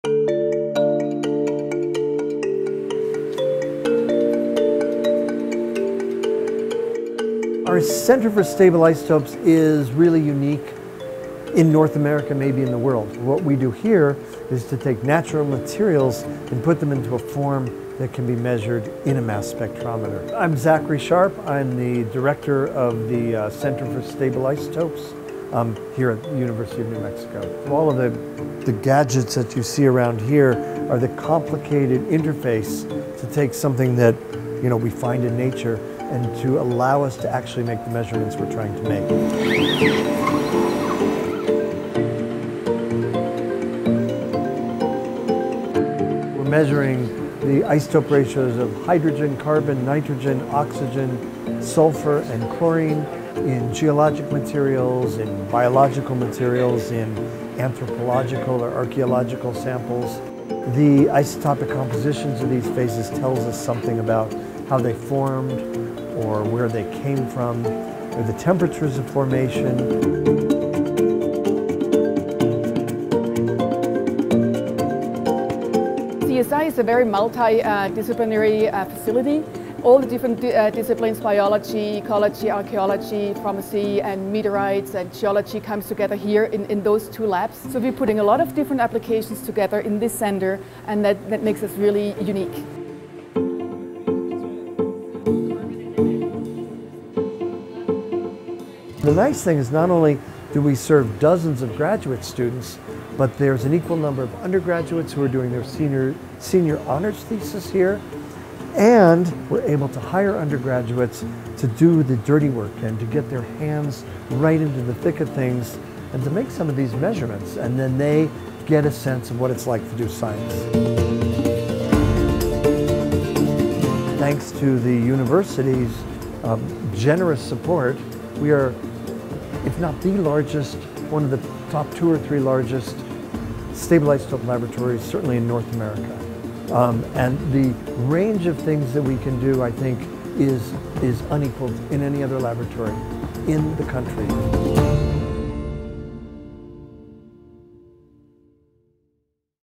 The University of New Mexico--Center for Stable Isotopes
The University of New Mexico’s Center for Stable Isotopes supports state-of-the-art, world-class, stable isotope research conducted by scientists and students across disciplines from Earth and Planetary Sciences, Biology, Anthropology, Chemistry and the Biomedical Sciences. Founded in 2014, the non-profit research focused laboratory and analytical facility is capable of measuring stable isotope ratios of light elements including carbon, nitrogen, oxygen, hydrogen, sulfur and chlorine.

Produced by Aaron Hilf